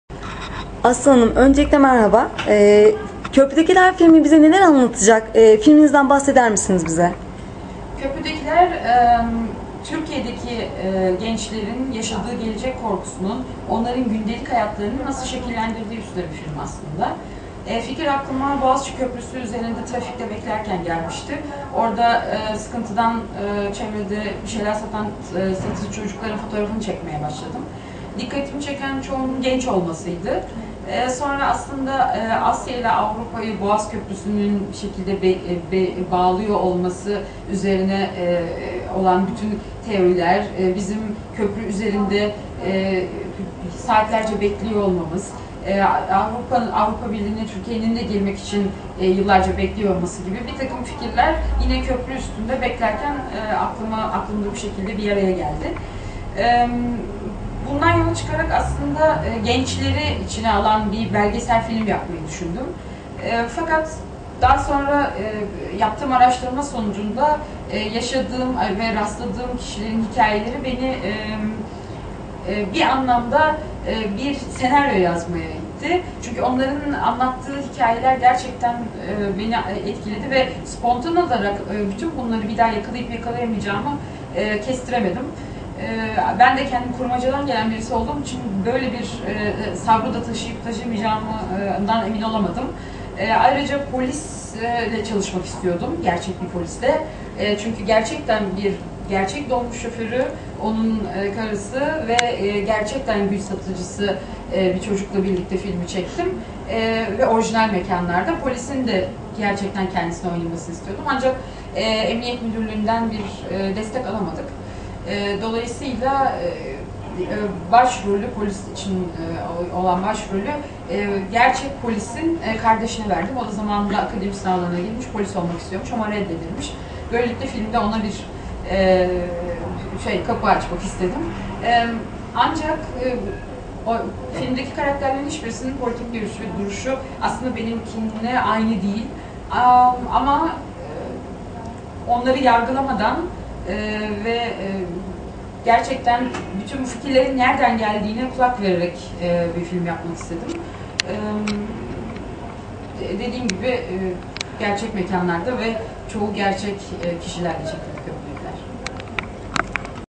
Aslı Özge röportajı - Köprüdekiler
19 Mart 2010'da vizyona giren Köprüdekiler filminin yönetmeni Aslı Özge ile yapılan çok özel röportaj.